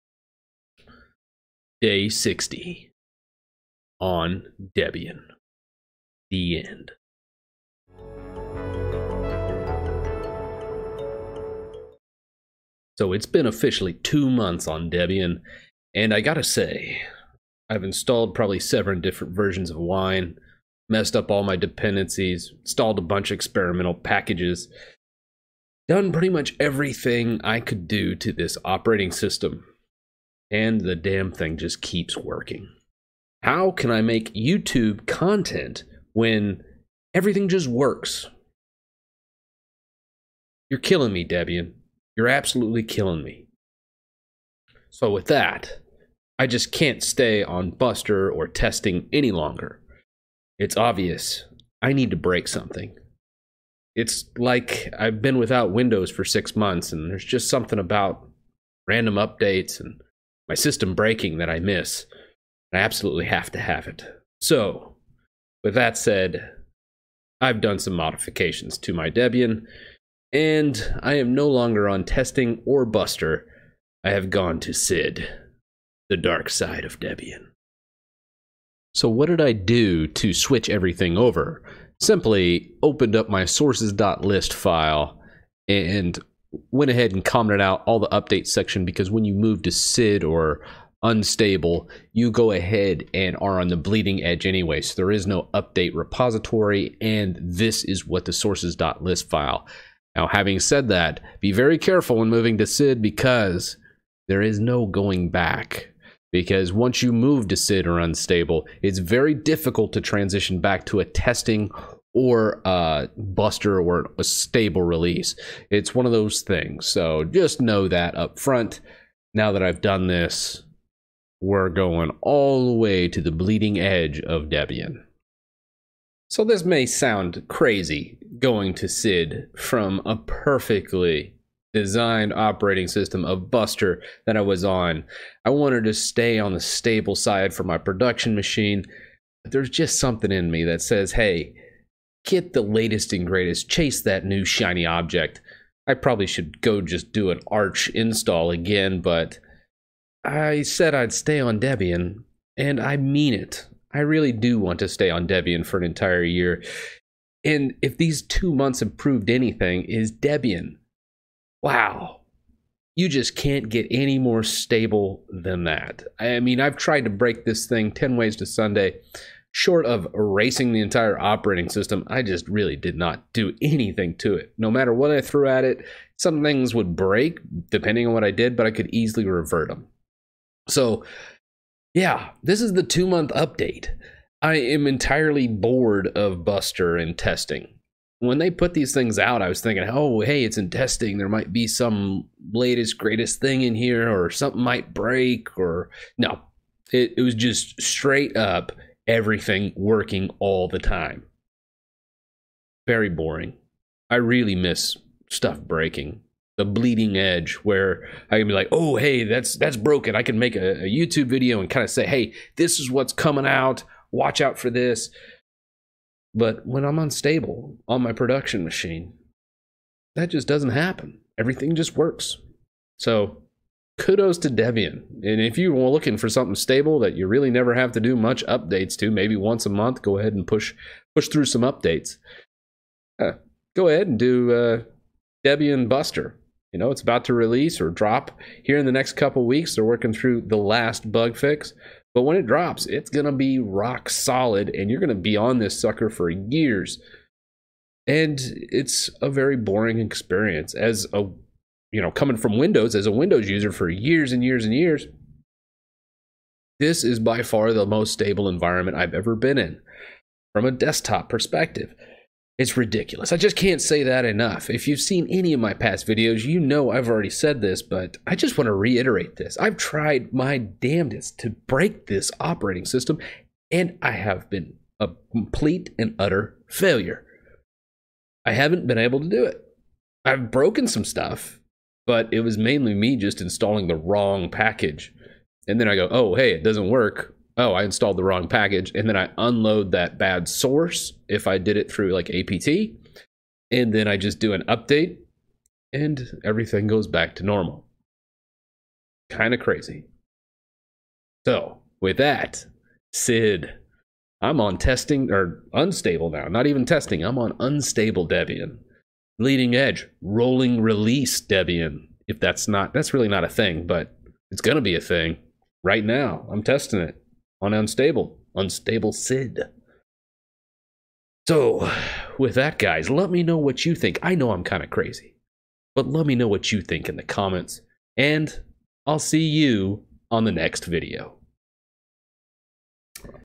Debian Linux - Day 60 | The End
I said I was doing a year of Debian Linux and on Day 60 we have reached the end of the road... .